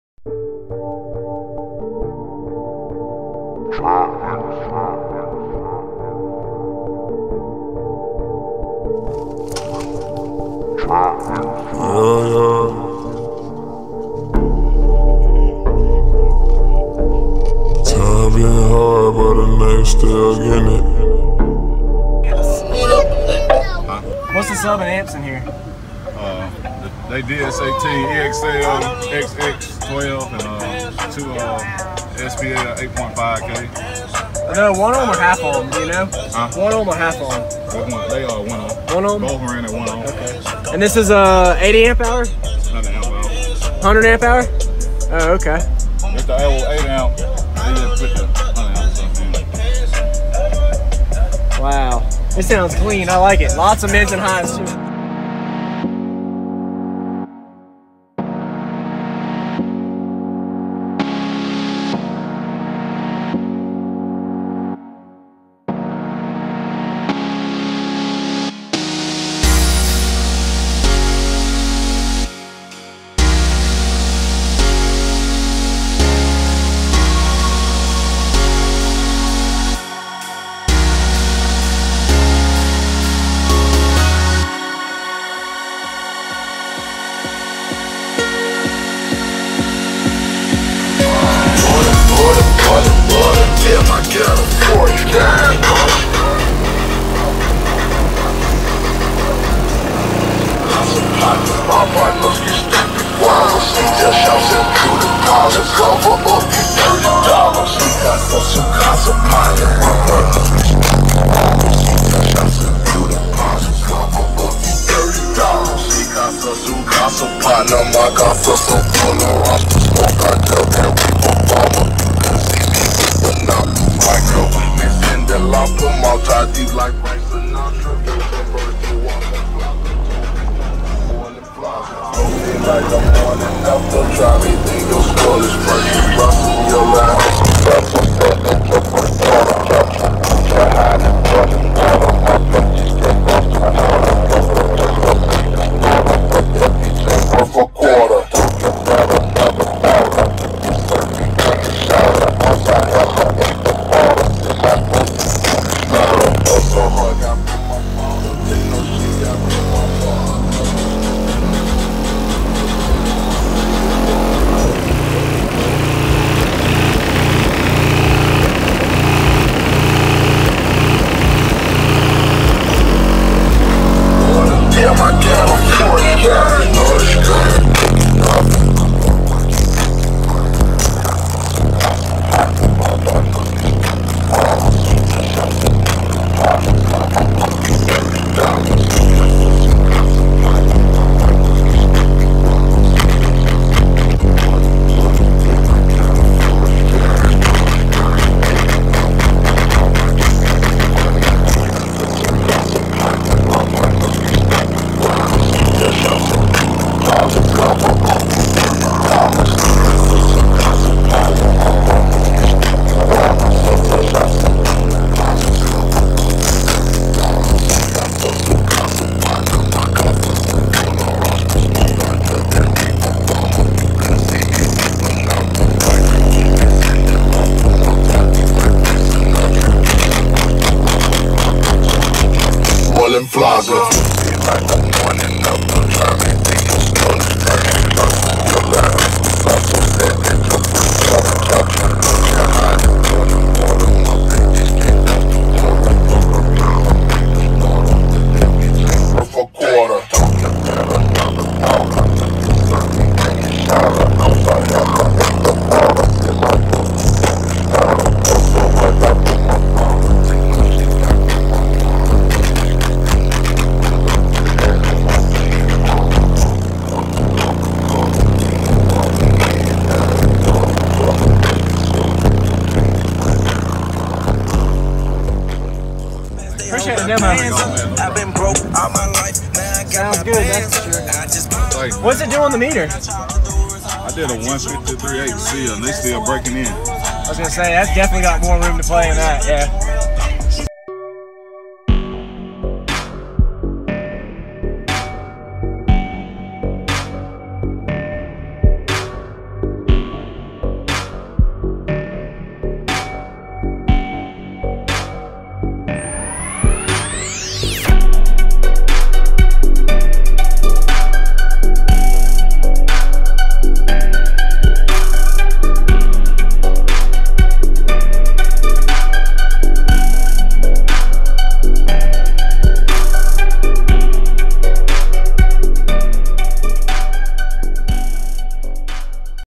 8 12S IN A TAHOE SLAPPIN
Tahoe with 8 12s showing off it's bump at a car show flexing everything. This setup gets down for sure.

No Wall Setups are always easier to get video footage of because everything flexes and you got more area to record.  This tahoe was playing low and loud with a great demo for sure.

DS18 EXL-X12.2D Extremely Loud 12-Inch 2000 Watts Competition Subwoofer with Dual 2-Ohms Voice Coil, DS18 12s EXL series
DS18 PRO-SPL8.5K1 8,500 Watts RMS SPL Subwoofer Class D Monoblock Amplifier
Clean Plexiglass style Rack
DS18 Mids and DS18 Highs supertweets
DS18 2 Channel Amp, DS18 4 Channel Amp
100AH Limitless Lithium Power
Metered 153dB+ Sealed on Dash with Subs brand new still breaking in.
8 12s in a SUV, eight 12s
2018 National Call Out Supershow